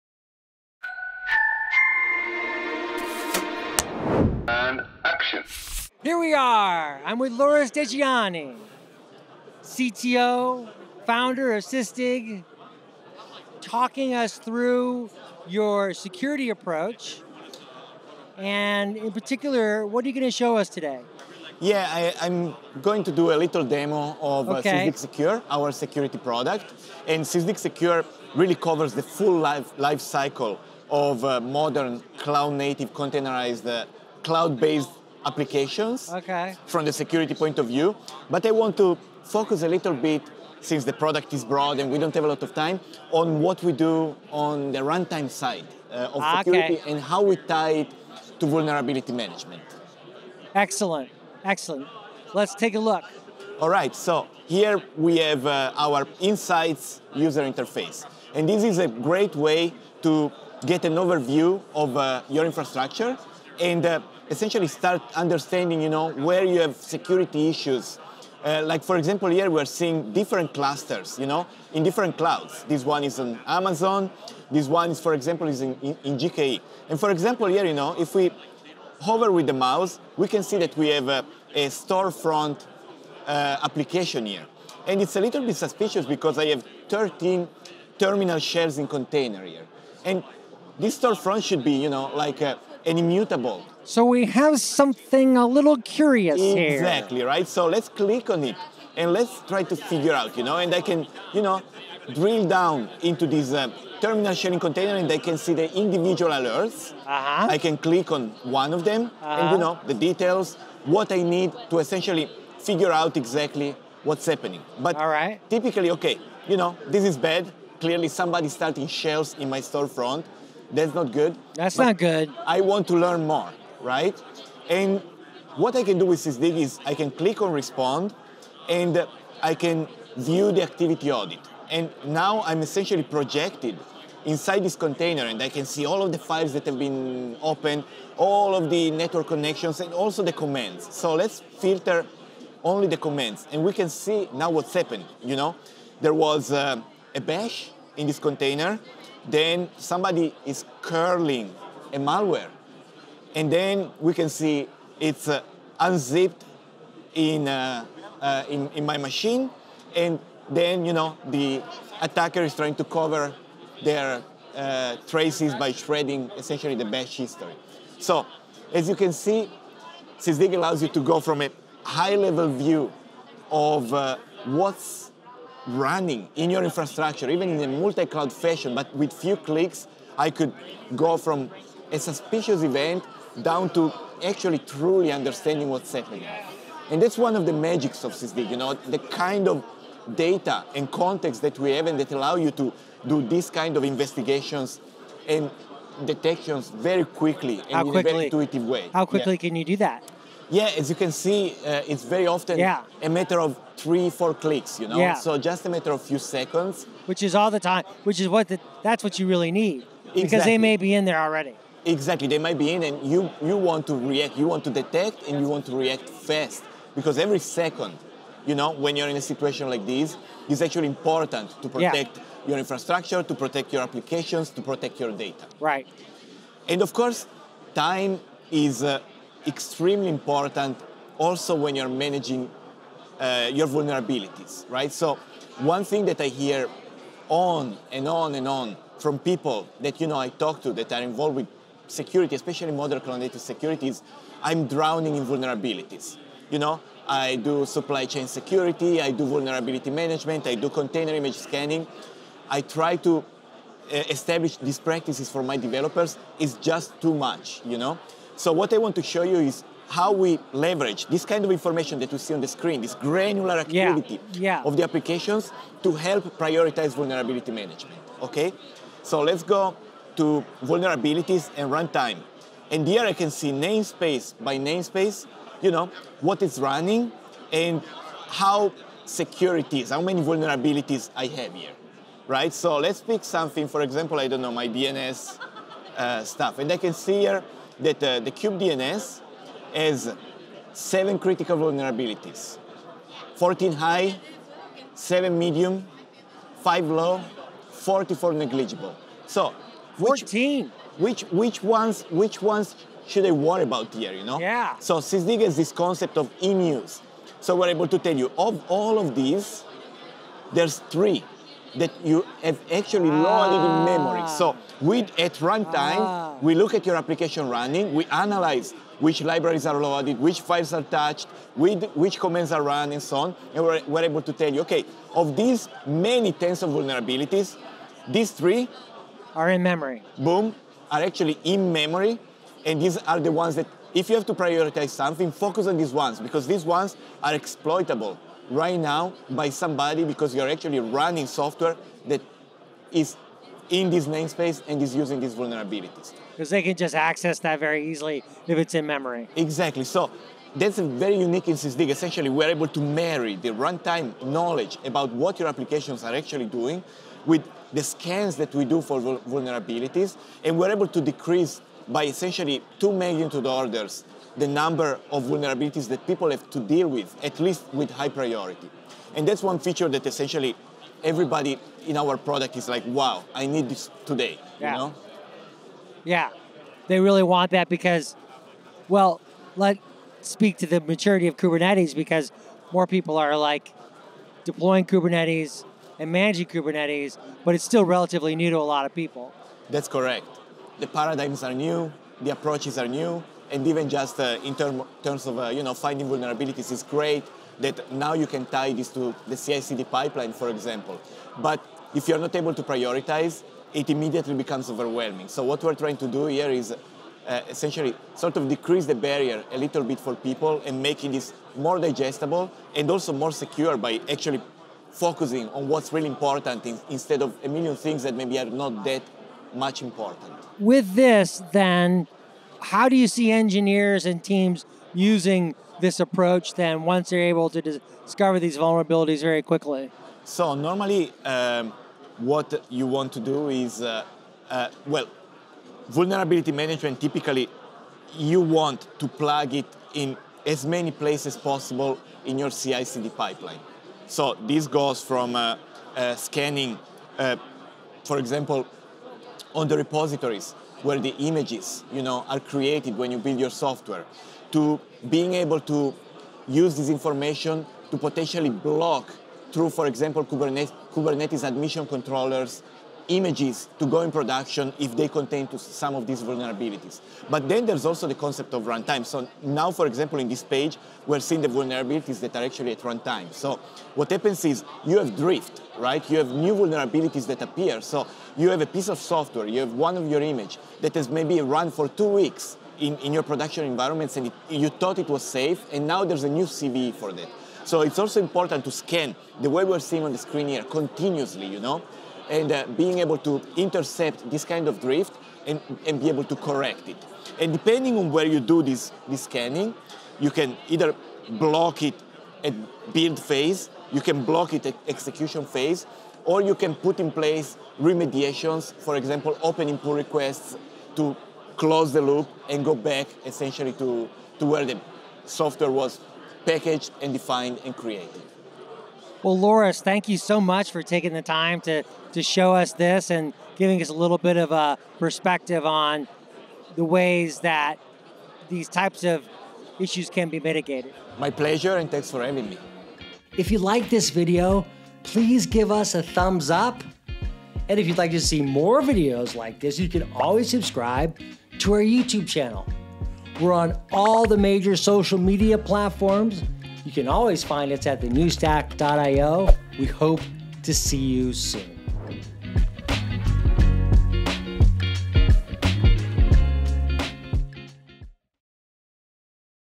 Demo: Sysdig Secure - Focus, Find and Fix Cloud Security Threats That Matter
Sysdig Secure really covers the full life cycle of modern cloud native, containerized cloud-based applications from the security point of view, according to Loris Degioanni, chief technology officer and founder of Sysdig. However, for this episode of The New Stack Demos, recorded at CloudNative SecurityCon in February, he opted to focus on only one aspect of the product. This demo, he said, concentrated on “how we tie it to vulnerability management.”

More people are deploying and managing Kubernetes, but it's still relatively new to a lot of people. Degioanni described how the paradigms are new, the approaches are new and different in terms of finding vulnerabilities, and how Sysdig Secure can bring vulnerablity management to the CI/ CD pipeline, for example, without it being too overwhelming.

“So what we're trying to do here is essentially sort of decrease the barrier a little bit for people and make this more digestible … and more secure by actually focusing on what's really important,” he said.

Check out Degioanni’s demo and learn more about how Sysdig Secure finds and prioritizes vulnerabilities.

Also, read The New Stack’s featured article about how the Sysdig Threat Research Team uncovered an extensive crypto mining operation, PurpleUrchin, which abuses free continuous integration and deployment service accounts. “PurpleUrchin: GitHub Actions Hijacked for Crypto Mining”